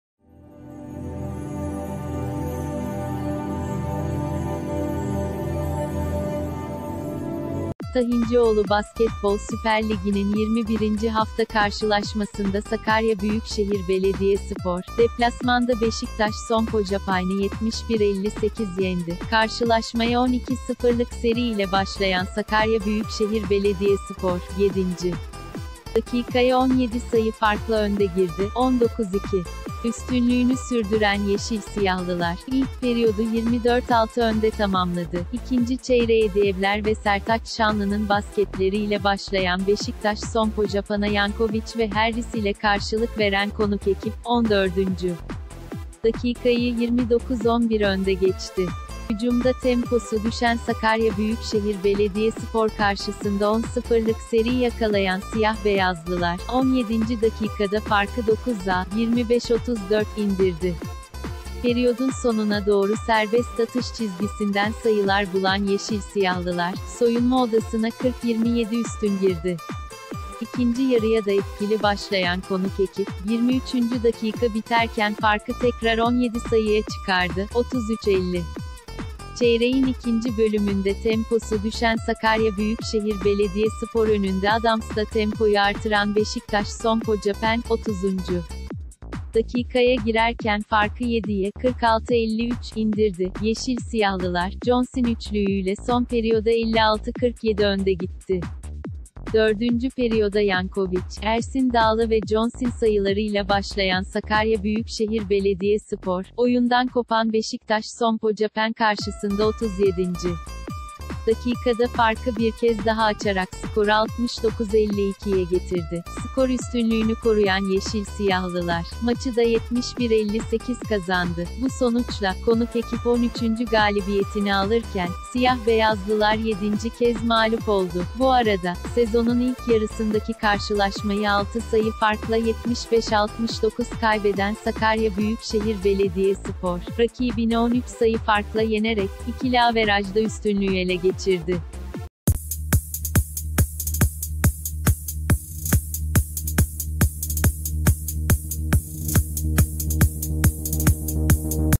Son Dakika Haberleri | Beşiktaş Sompo Japan evinde darmadağın
Tahincioğlu Basketbol Süper Ligi'nin 21. hafta karşılaşmasında Sakarya Büyükşehir Belediyespor, deplasmanda Beşiktaş Sompo Japan'ı 71-58 yendi. Karşılaşmaya 12-0'lık seri ile başlayan Sakarya Büyükşehir Belediyespor, 7. dakikaya 17 sayı farkla önde girdi: 19-2. Üstünlüğünü sürdüren yeşil-siyahlılar, ilk periyodu 24-6 önde tamamladı. İkinci çeyreğe Diebler ve Sertaç Şanlı'nın basketleriyle başlayan Beşiktaş Sompo Japan'a Jankovic ve Harris ile karşılık veren konuk ekip, 14. dakikayı 29-11 önde ge...